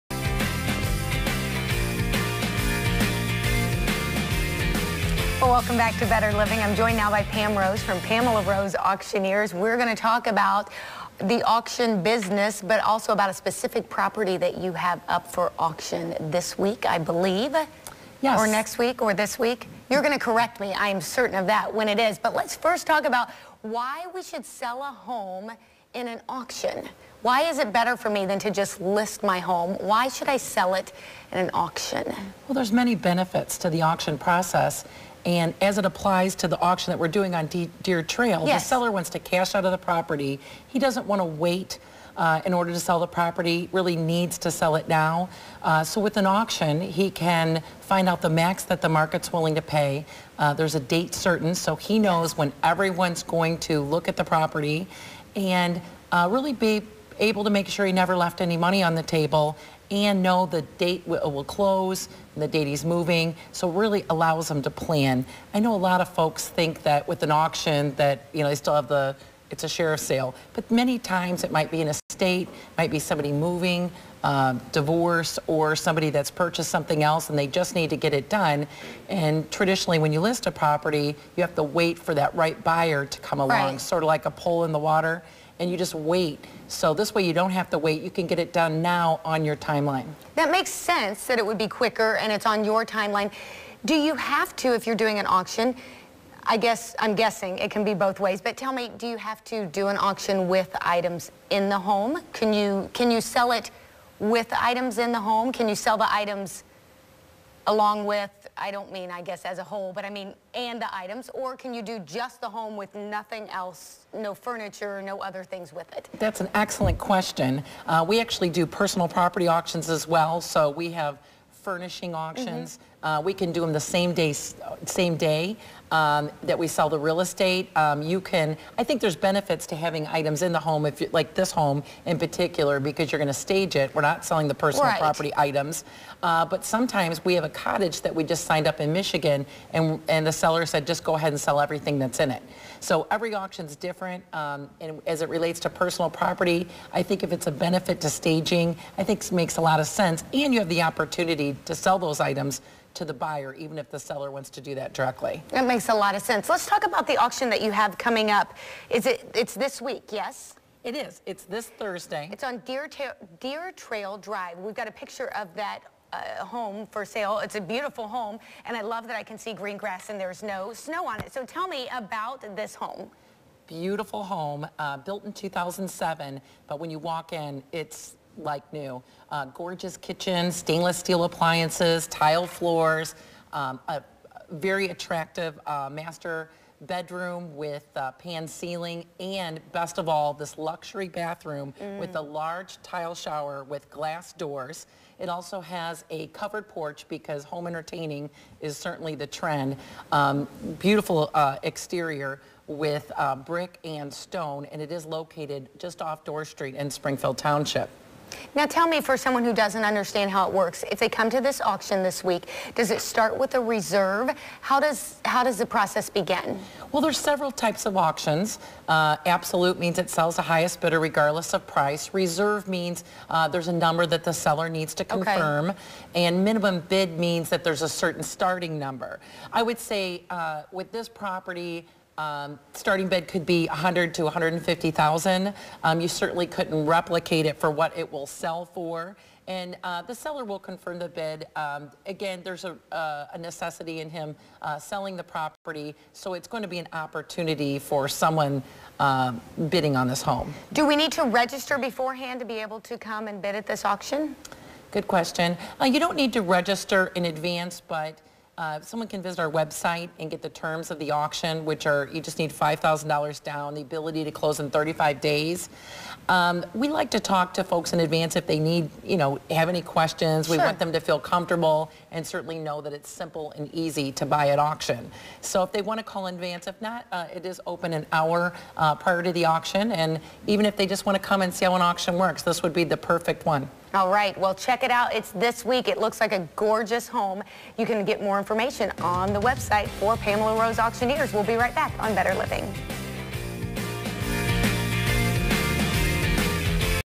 Better Living: Pamela Rose Auction Company
Why should you sell your home at auction? Pamela Rose tells Better Living.